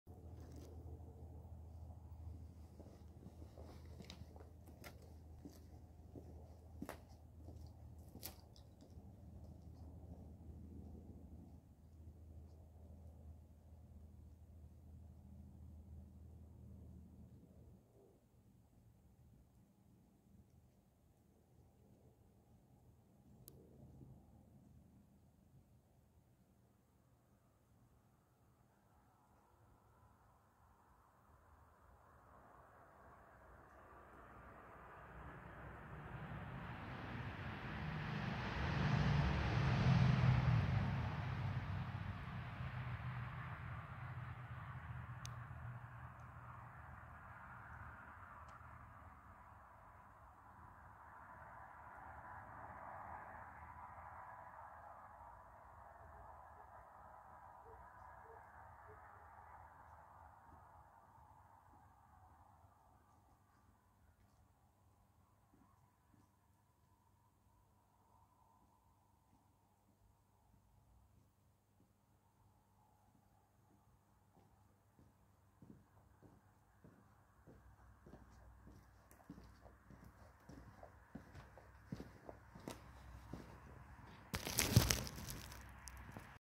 the walking Uli
100m bis zum Tor geht jetzt auch Abends ohne 🔦 und Tor und Stellplatz sind Taghell beleuchtet